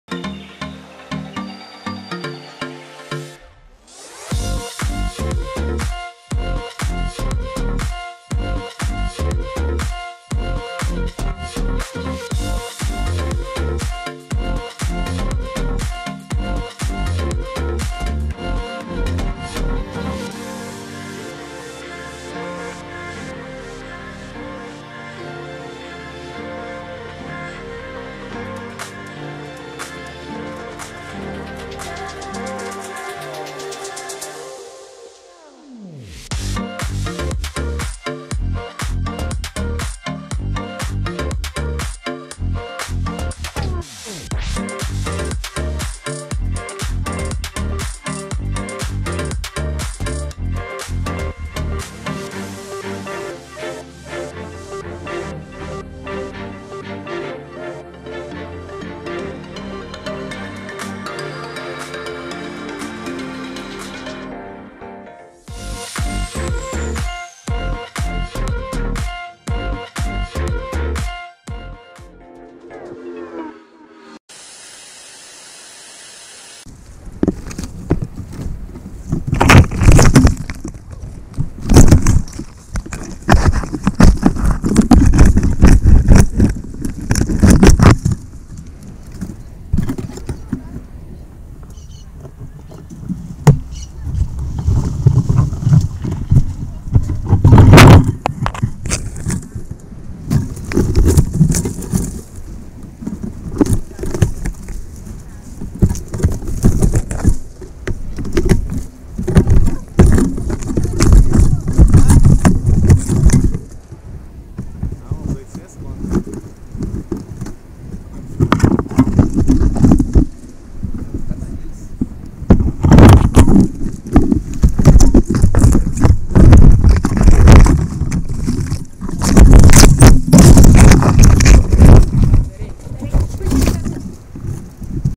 Treinando algumas manobras (Deu ruim!)
Treinando algumas manobras e em um power loop acho que árvore cresceu um pouco kkkkk.

Comenta aí!

Radiomaster TX16s
Bateria Radiomaster
Bateria 6s 1300
Bateria 4s 1500